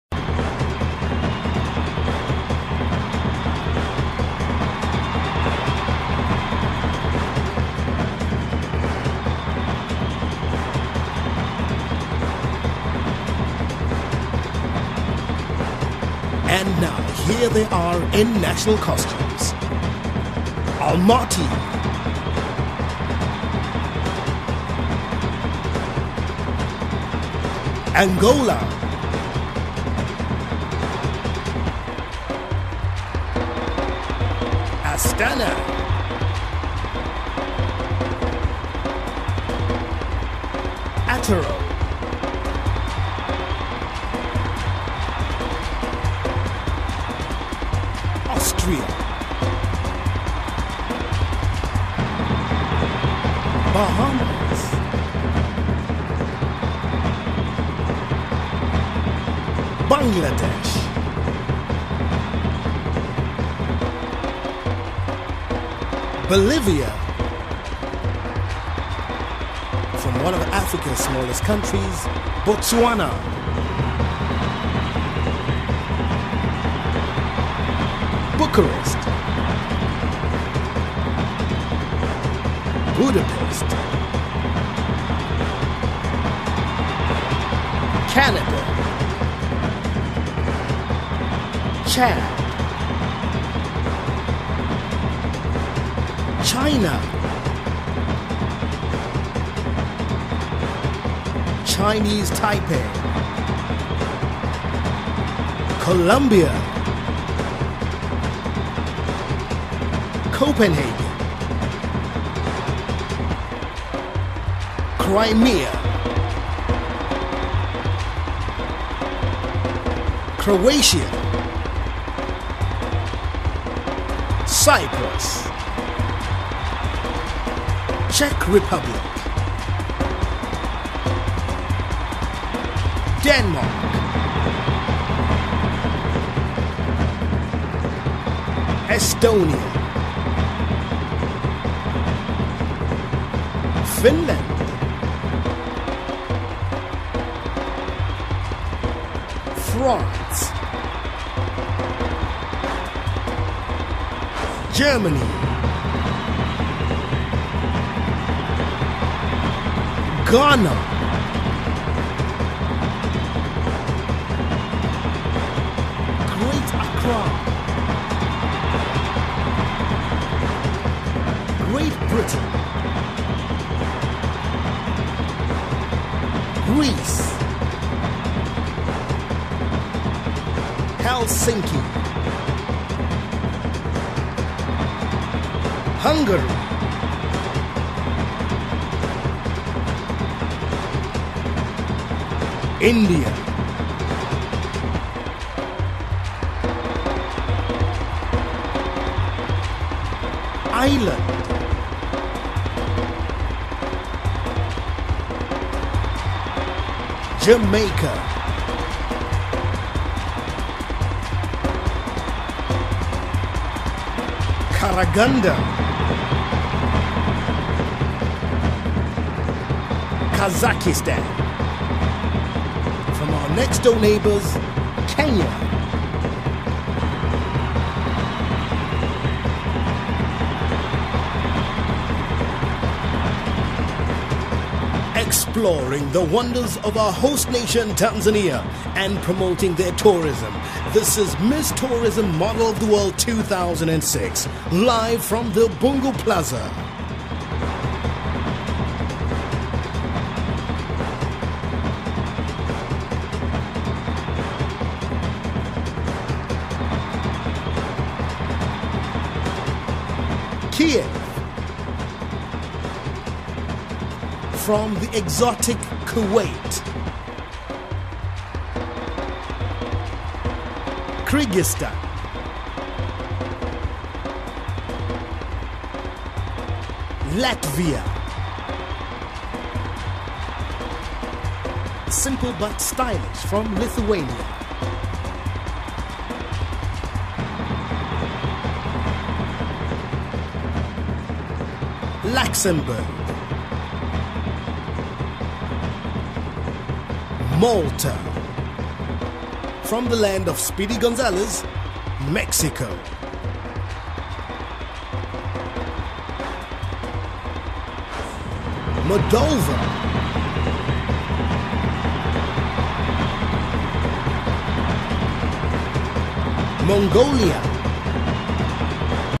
Model of the world Tanzania
One of OilinSha's most well trained models was awarded the opportunity to represent the Bahamas at the Top Model Of The World in Tanzania. Tessa Simone Lightbourne was first discovered and trained by OilinSha and given the opportunity because of her potential as an international quality